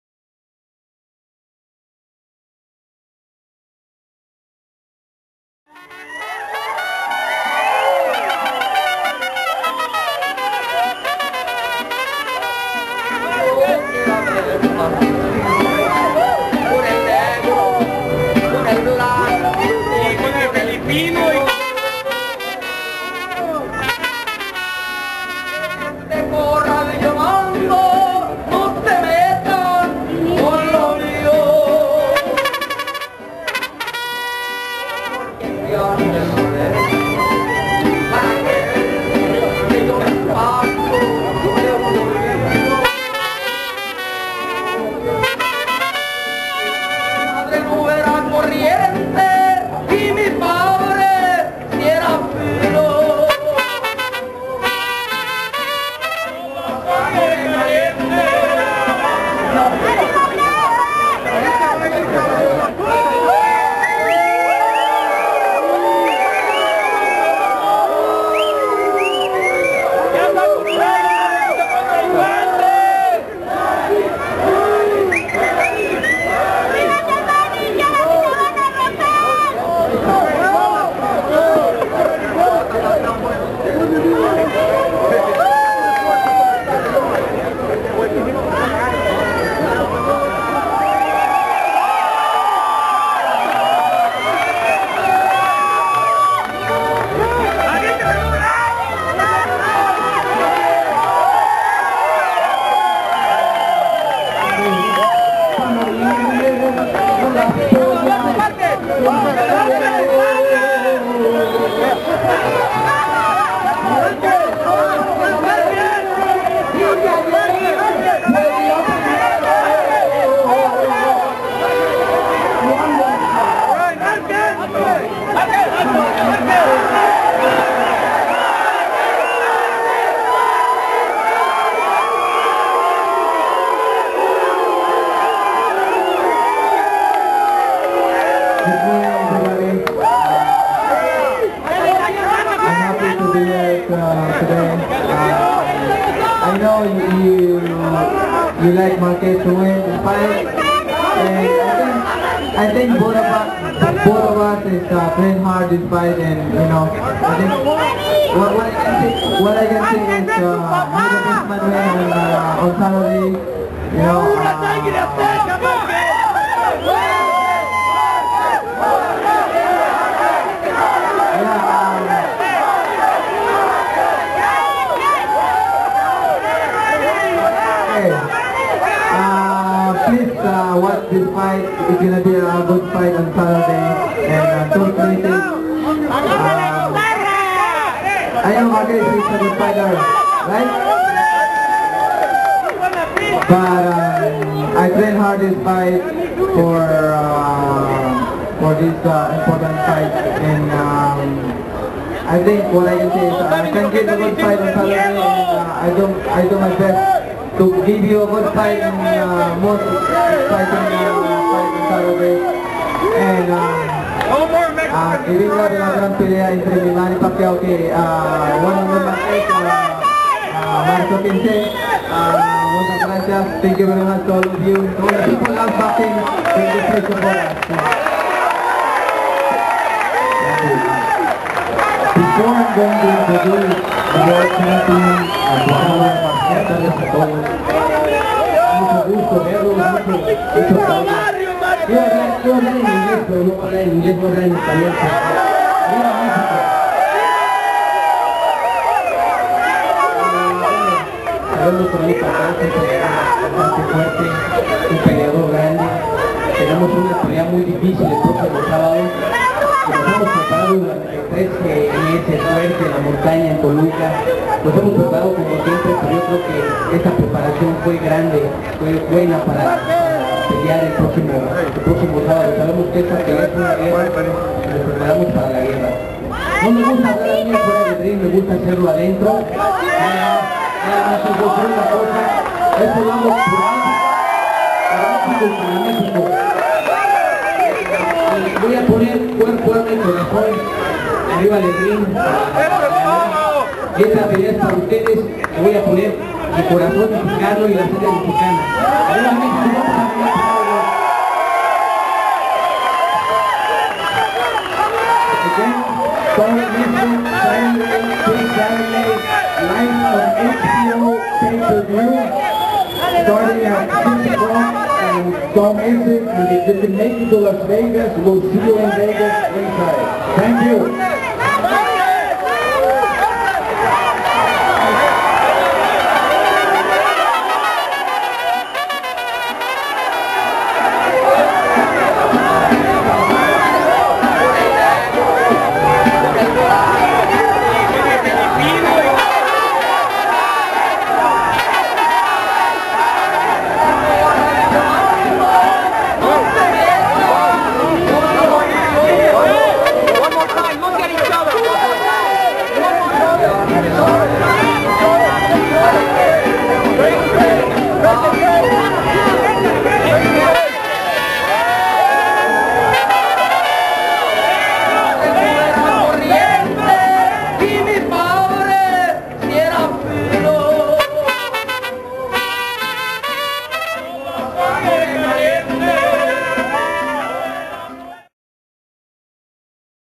Marquez vs. Pacquiao II Final Press Conference
Juan Manuel Marquez vs. Manny Pacquiao II Final Press Conference in Los Angeles for their fight on Saturday, March 15 at Mandalay Bay in Las Vegas